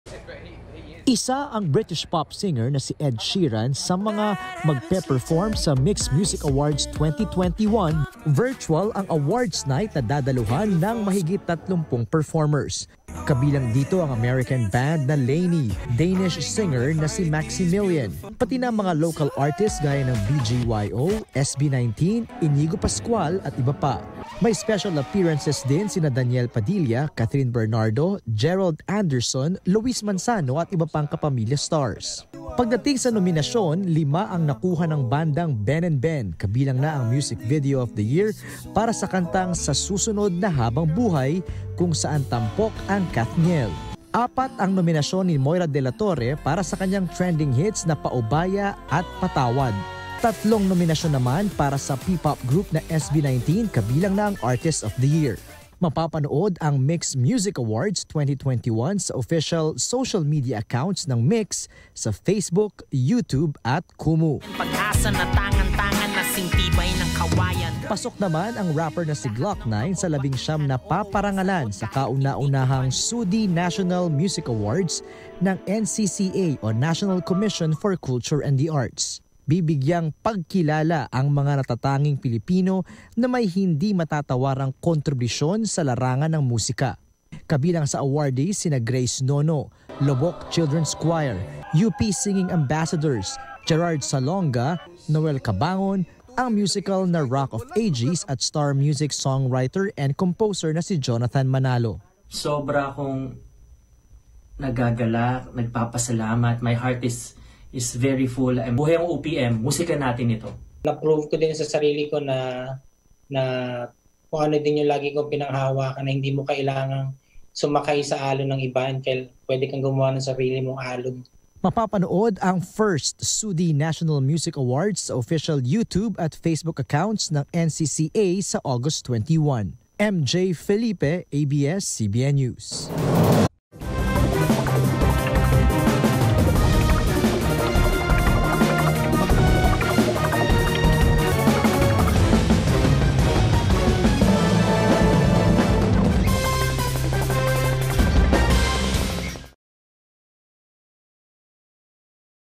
LANY, Ed Sheeran magpe-perform sa virtual MYX Music Awards | Star Patrol
Magpe-perform ang American band na LANY at si Ed Sheeran sa MYX Music Awards gabi ng August 7.